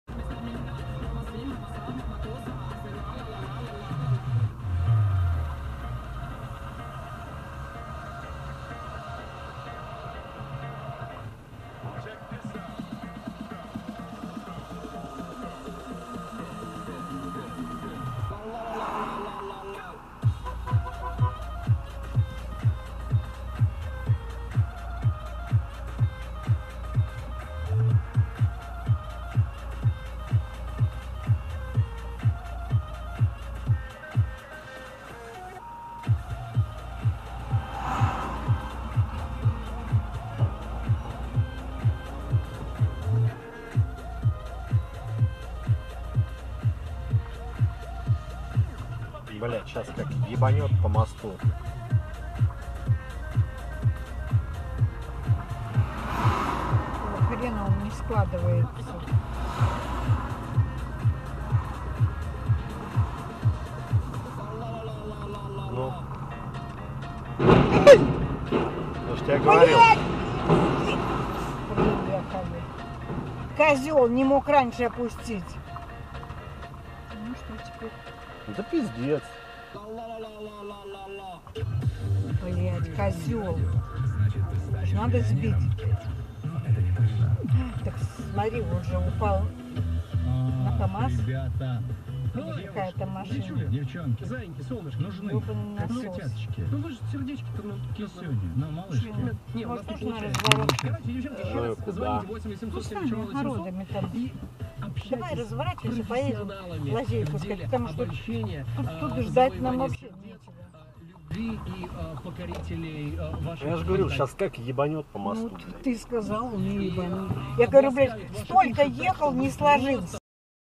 Dump truck demolishes pedestrian bridge || Viral Video UK
October 11th, 2020, Russia, Lyuban city. ''A dump truck demolished a pedestrian bridge at 614 km of the M-10 "Russia" highway in Lyuban in the Tosno district of the Leningrad region on Sunday. No pedestrians were hurt either. Only 49-year-old Volvo dump truck driver sustained moderate injuries.''